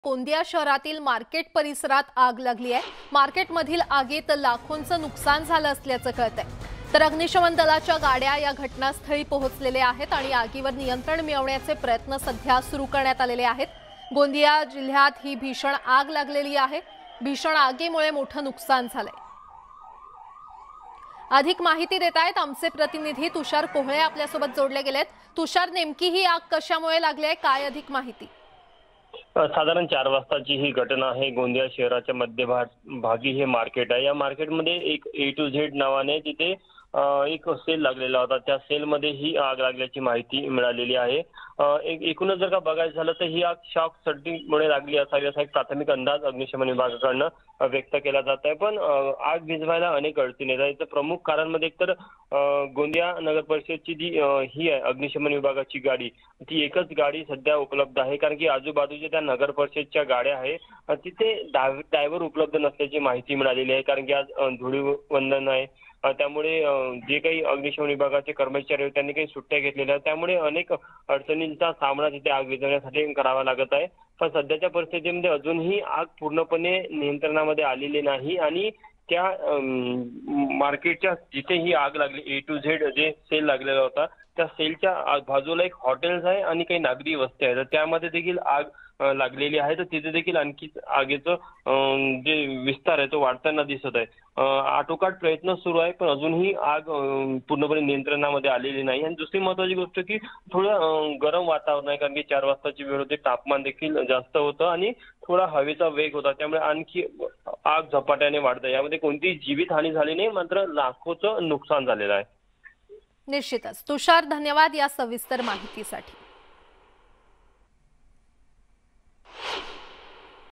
Gondia Fire : गोंदिया शहरातील मार्केट परिसरात भयंकर आग, आगीत लाखोंचे नुकसान
गोंदिया शहरातील मार्केट परिसरात भयंकर आग लागली आहे. या आगीत लाखोंचे नुकसान झाले आहे. आगीवर नियंत्रण मिळवण्यासाठी प्रयत्न सुरू आहेत.